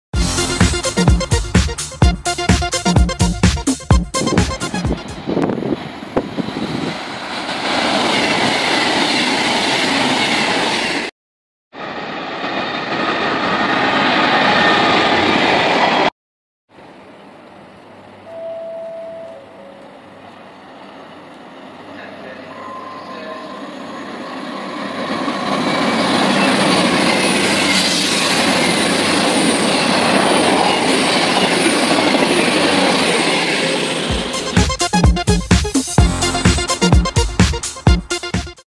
Two Virgin Voyagers and a Freightliner 66 at Stafford
A very quick change at Stafford where we see two Virgin Voyagers and a Freightliner 66.